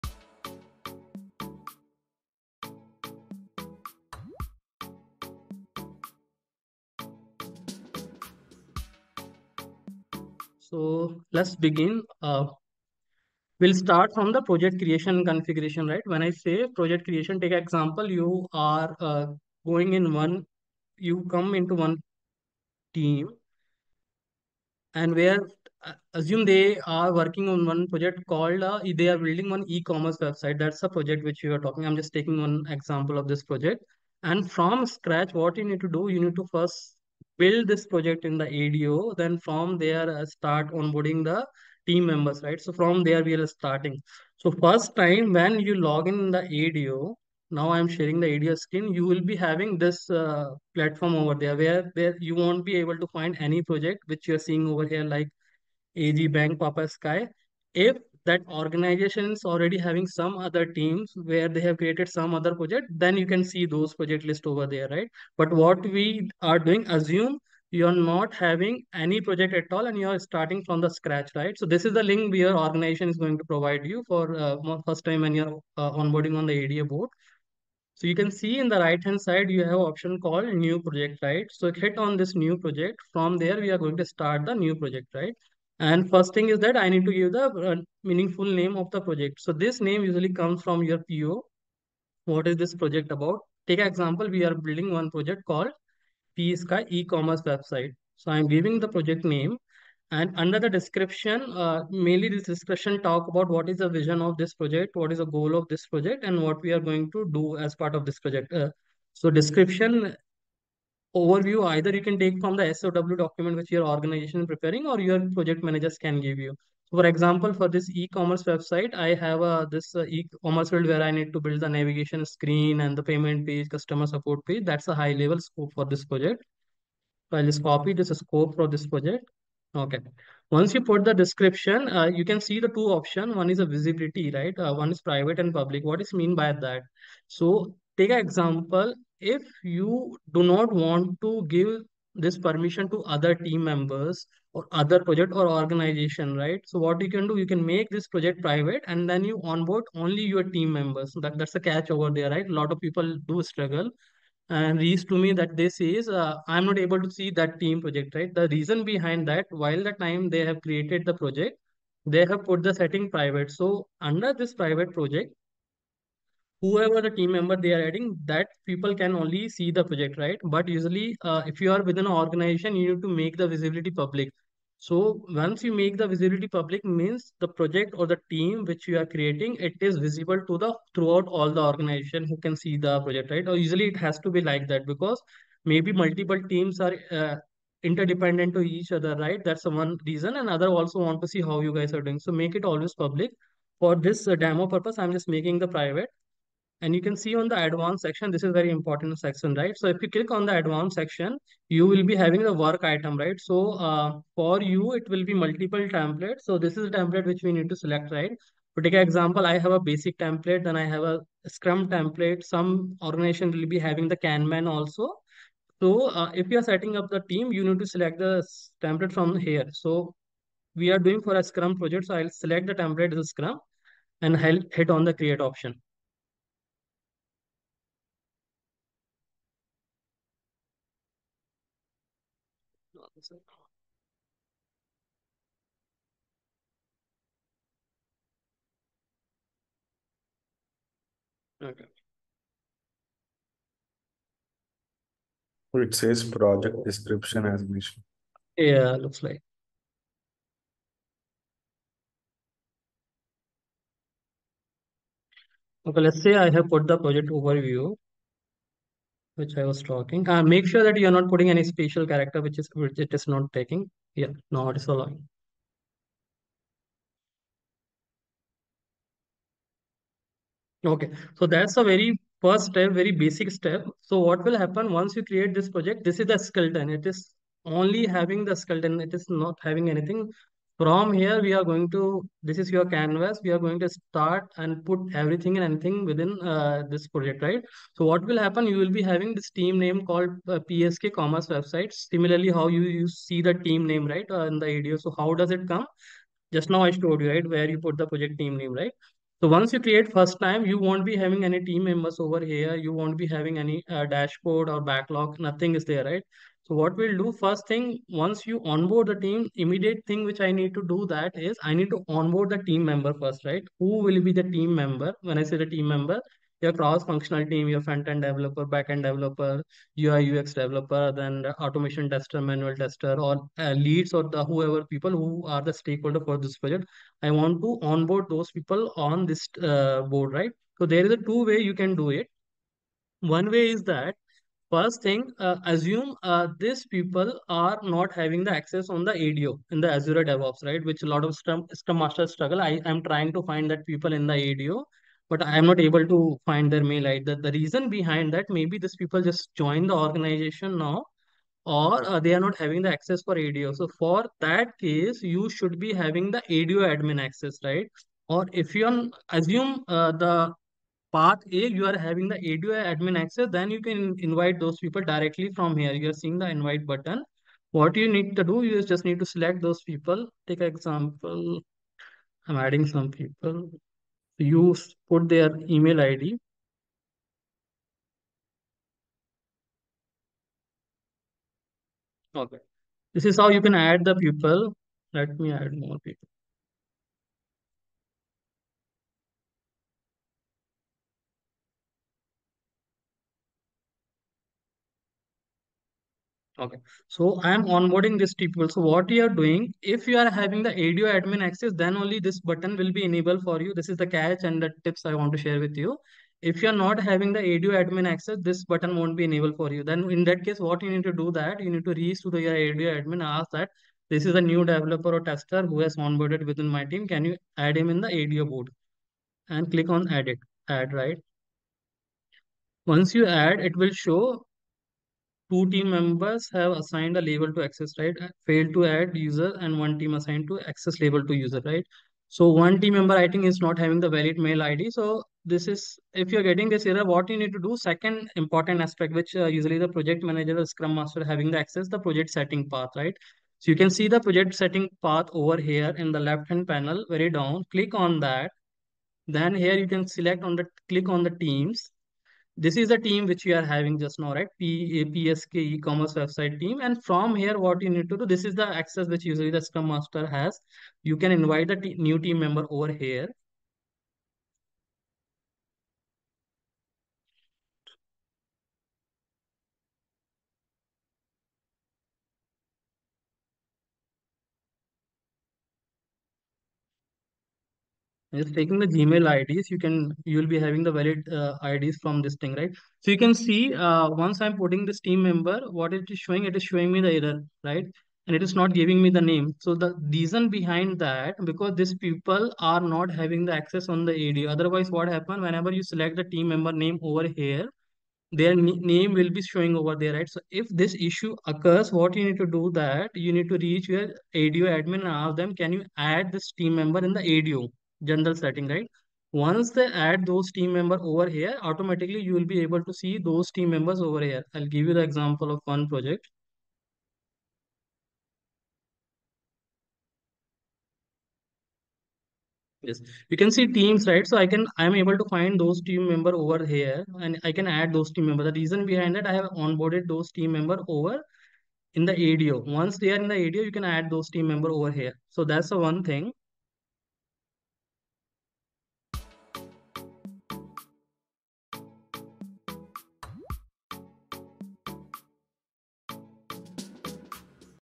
How to create project in Azure DevOps from Scratch
Title: How to Create a Project in Azure DevOps from Scratch

Description:
Join us in this quick guide to set up a new project in Azure DevOps. Learn the essentials of project creation, team collaboration, and tool integration. Ideal for beginners and those looking to refine their DevOps skills.

Call to Action:
💬 Share your Azure DevOps experiences in the comments.
🔔 Follow us for the latest tech tutorials.

Start your Azure DevOps journey today!